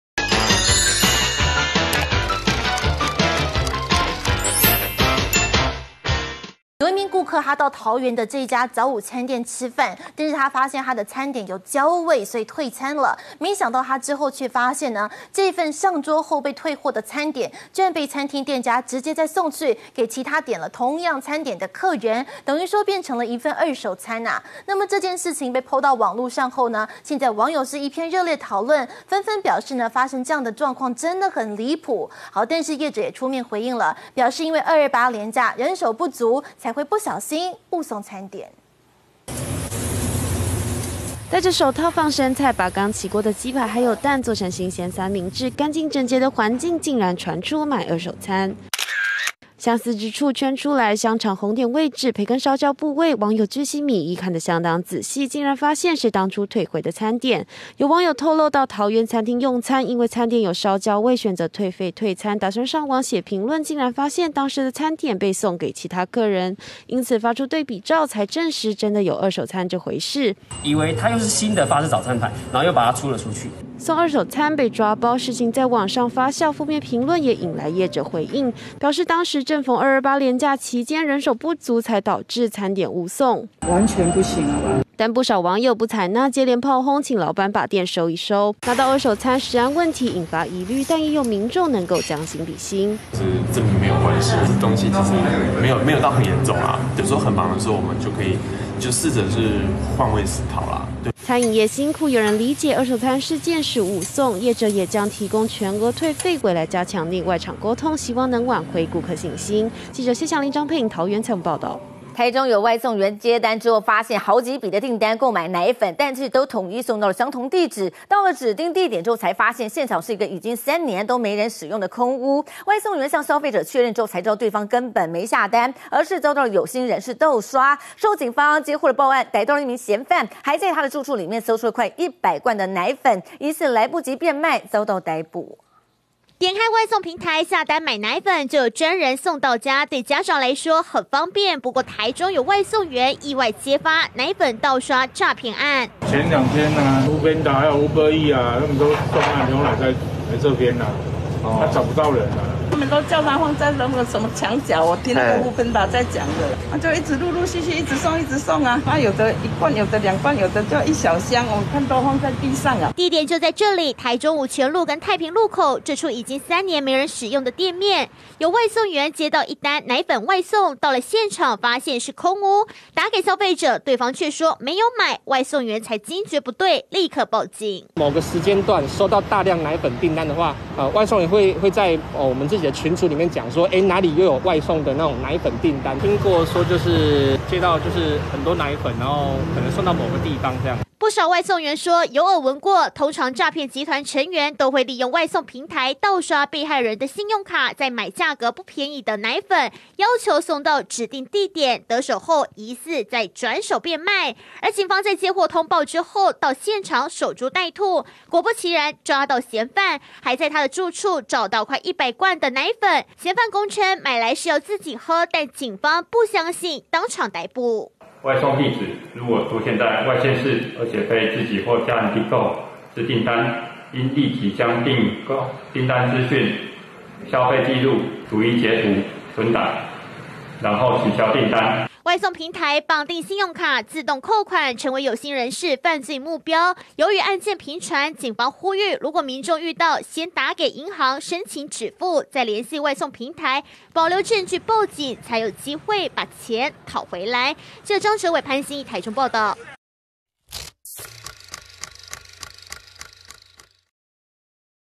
【新聞一點靈】食物焦遭退竟端別桌 業者喊冤「誤送」 狂訂奶粉送空屋 外送員揪盜刷詐團
一名顧客到桃園一家早午餐店用餐，因為發現食物有焦味而退餐，想不到之後發現，這份被退貨的餐點，竟然被餐廳送去給其他點了同樣餐點的客人，成了一份二手餐。事情一PO上網後，引起網友一陣熱烈討論，紛紛表示發生這種情況相當離譜，但業者也出面回應，表示因為是228連假人手不足，才會不小心誤送餐點。

台中出現奶粉盜刷集團，有外送員接單發現，好幾筆訂單購買奶粉，但都統一送到相同地址，到指定地點卻發現，現場是一個已經三年沒人使用的空屋，外送員向消費者確認，才知道對方跟本沒下單，而是遭到有心人士盜刷，警方接獲報案抓到一名嫌犯，還在他的住處搜出約100罐的奶粉，疑似來不及變賣就被逮捕。